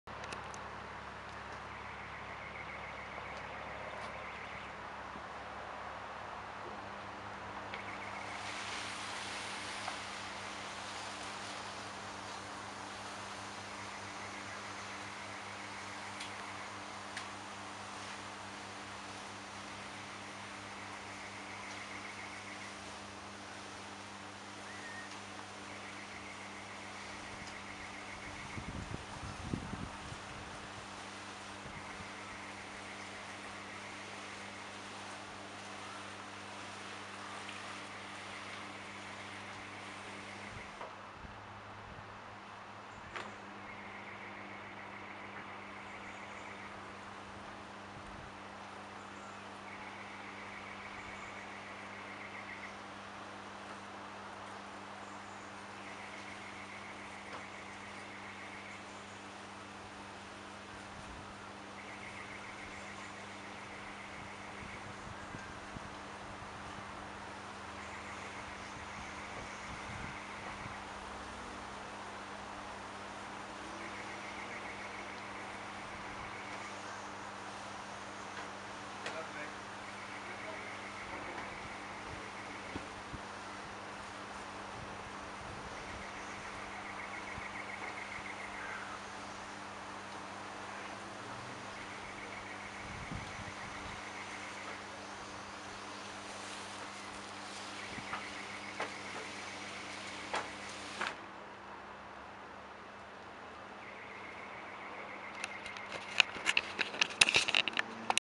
Coverstar Automatic Pool Safety Cover
Coverstar Automatic Pool Safety Cover - opening and closing.